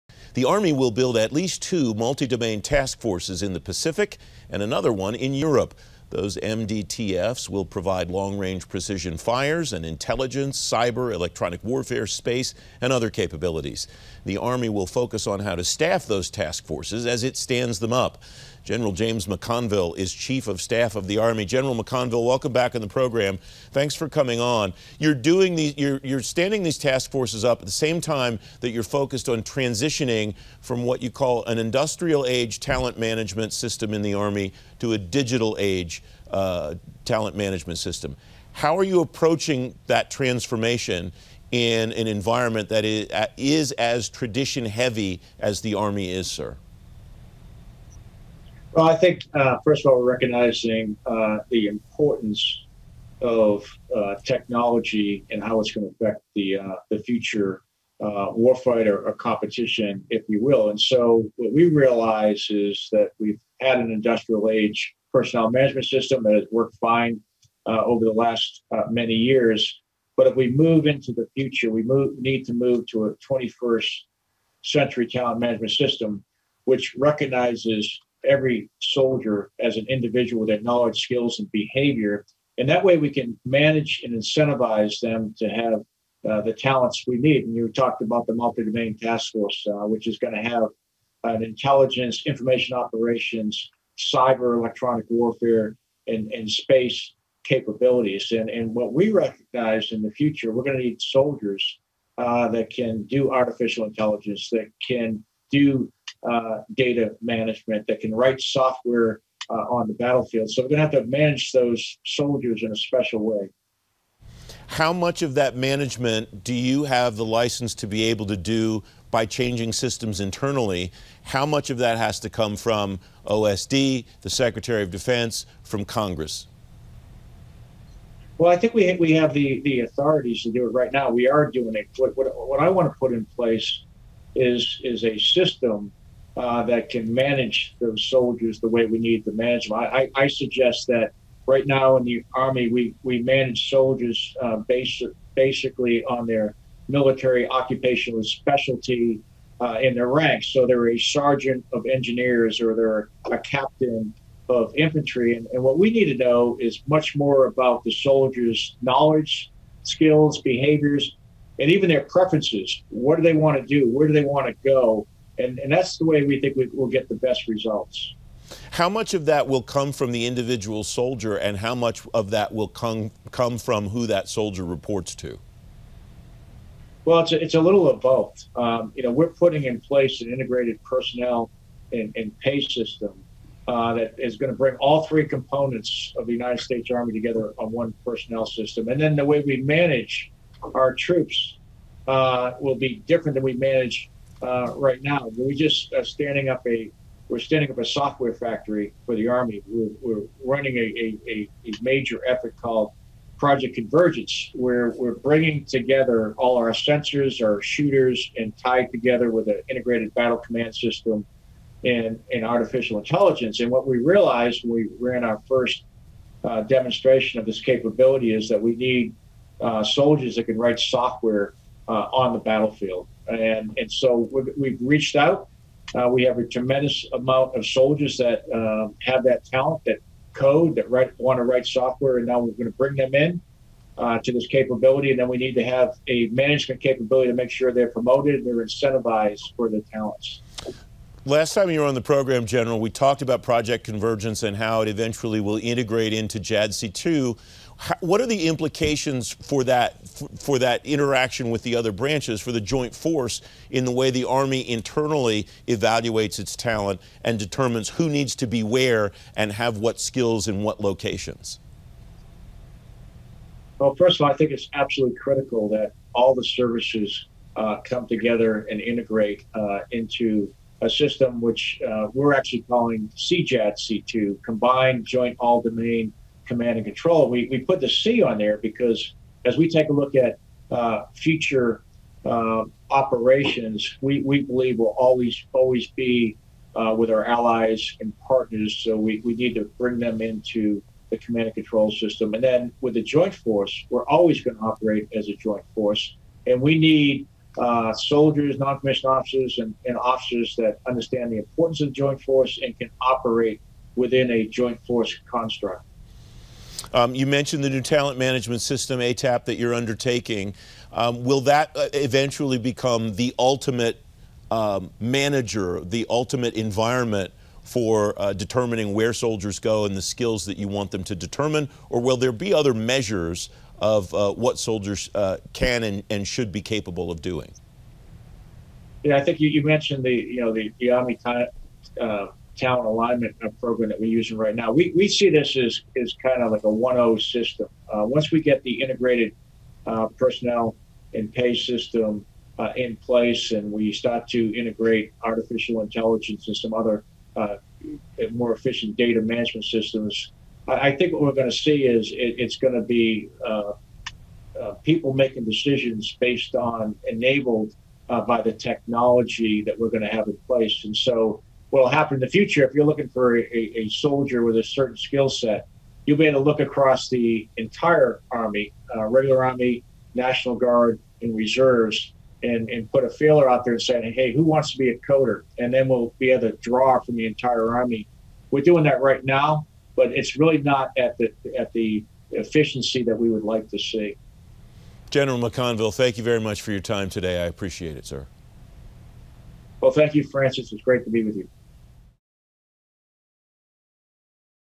Upgraded Army talent management system will meet 21st century demands
Gen. James McConville, Chief of Staff of the U.S. Army, discusses the upgrade to a new, “digital age” talent management system for the Army